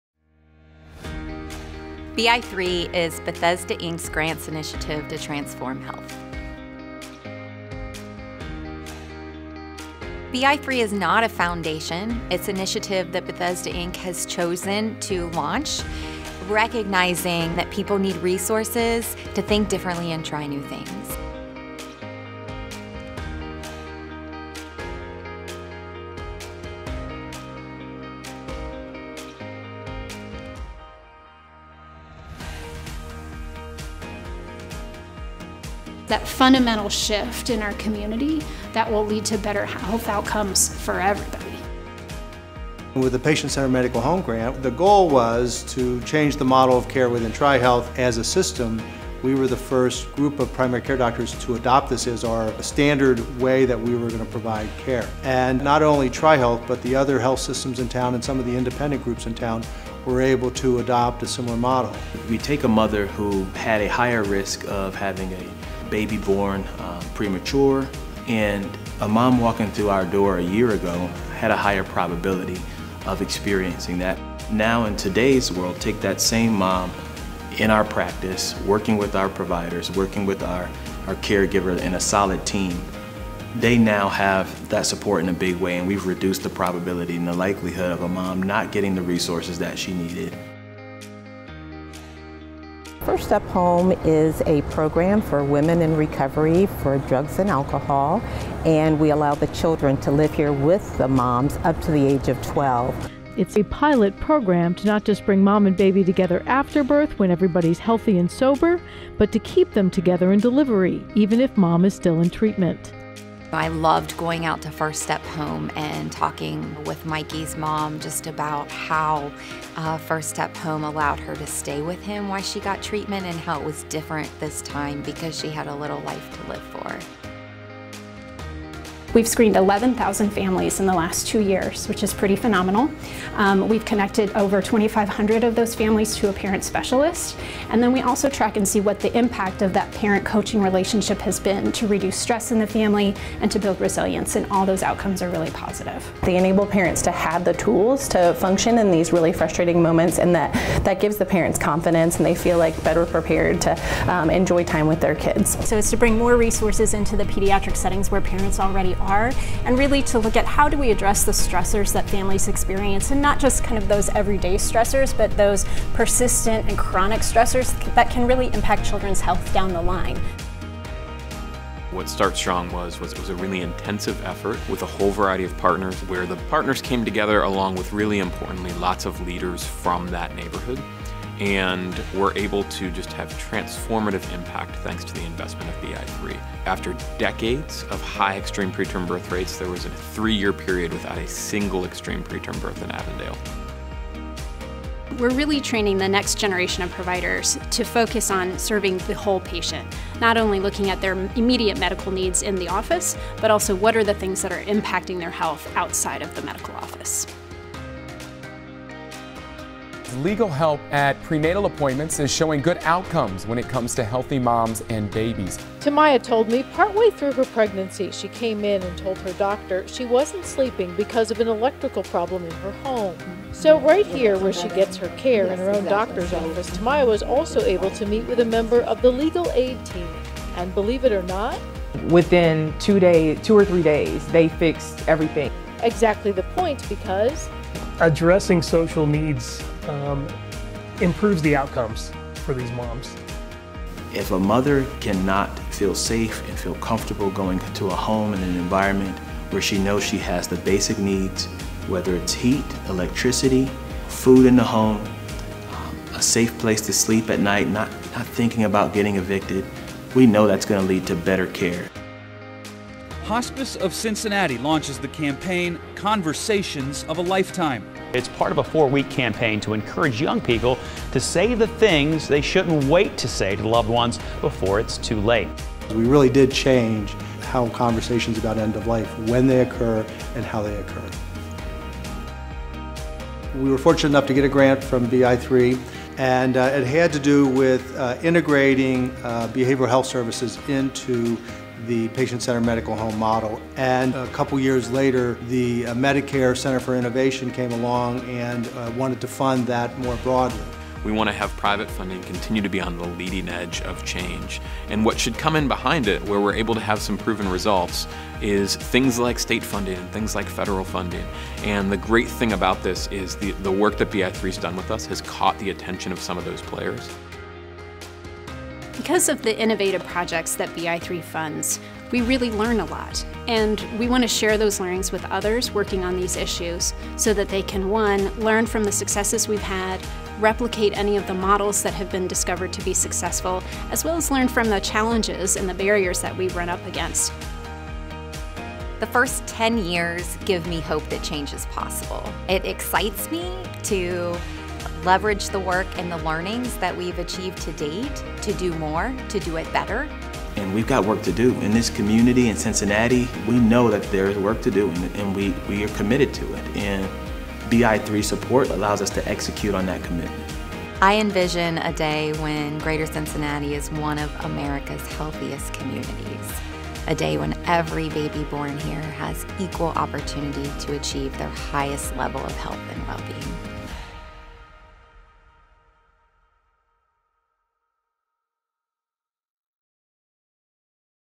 bi3 - 10 Years of Innovation
bi3 is celebrating 10 years of innovation. Through $48 million invested in TriHealth and the Cincinnati community, bi3 has funded more than 50 ideas pioneering new models of care — impacting thousands of lives and transforming health.